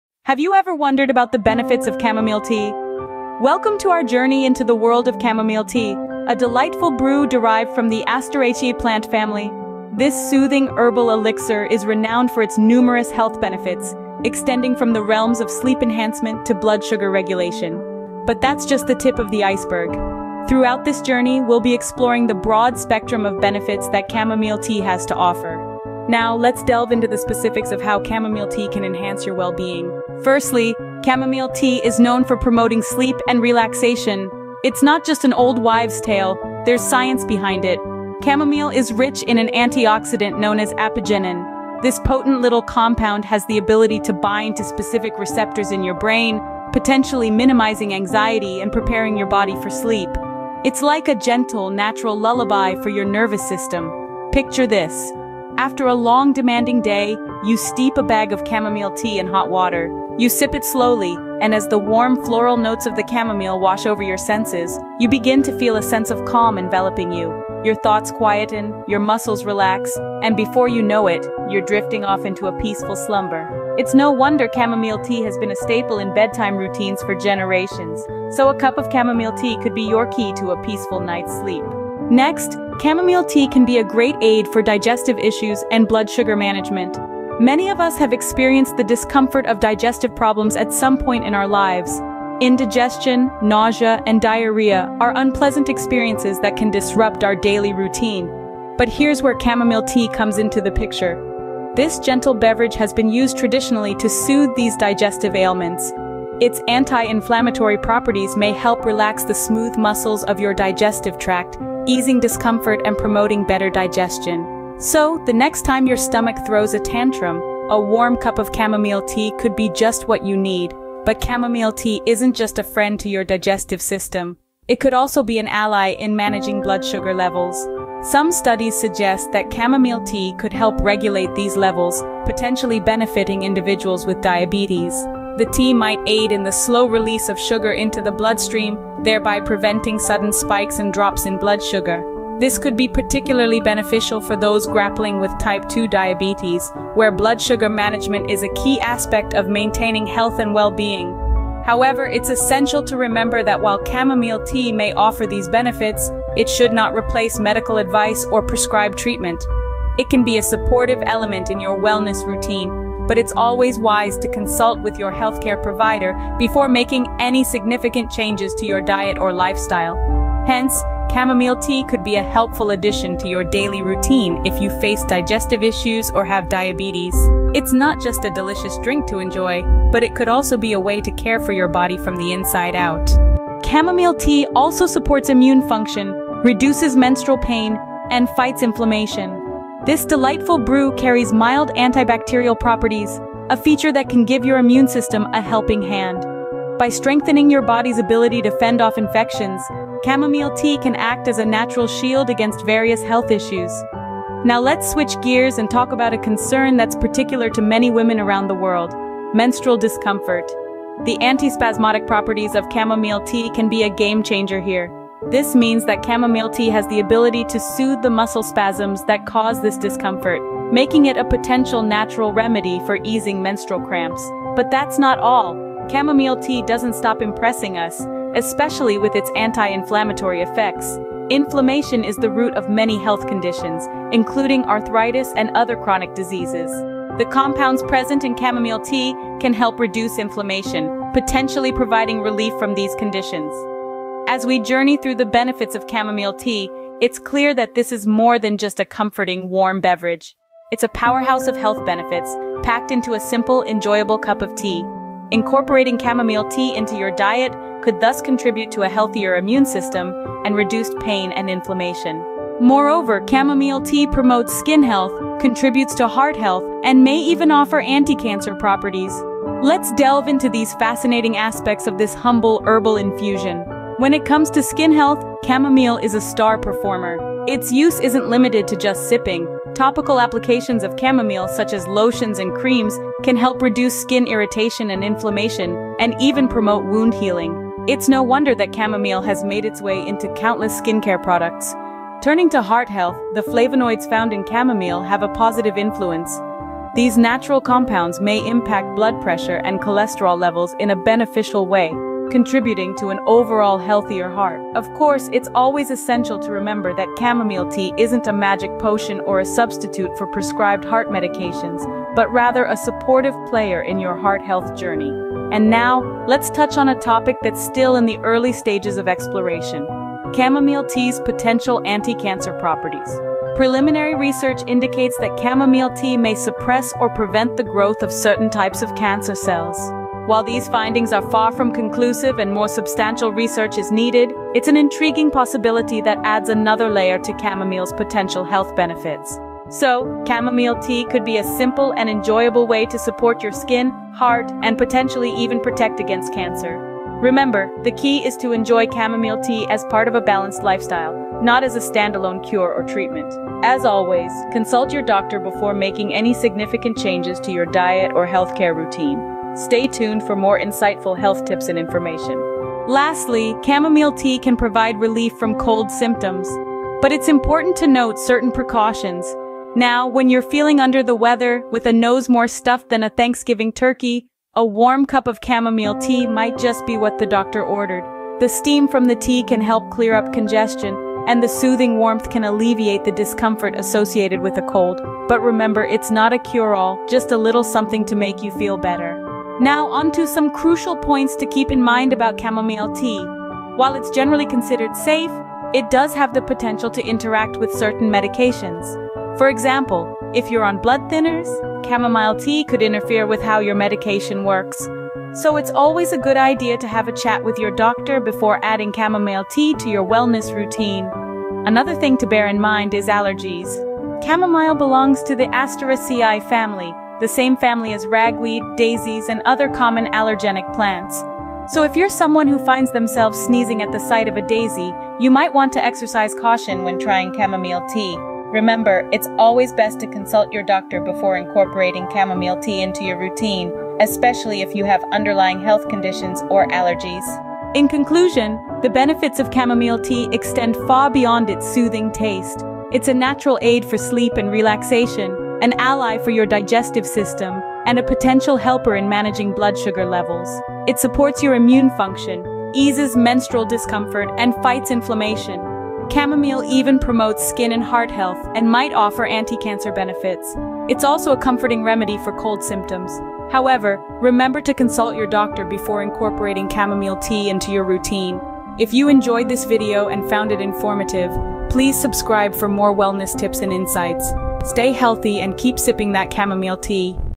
What are the advantages of having chamomile tea?
Dive into the enchanting world of chamomile tea with our latest video, "What are the advantages of having chamomile tea?" This captivating journey explores the history, benefits, and soothing properties of one of the most beloved herbal teas. From its use in ancient civilizations to its modern-day status as a magical elixir, chamomile tea offers a plethora of health benefits, including stress reduction, improved sleep, and digestive aid. Discover the reasons behind chamomile tea's enduring popularity and learn how to incorporate this wonder flower into your daily routine for a touch of tranquility. Whether you're a long-time enthusiast or a curious newcomer, this video will enlighten you on why chamomile tea is considered a must-explore wonder of the natural world.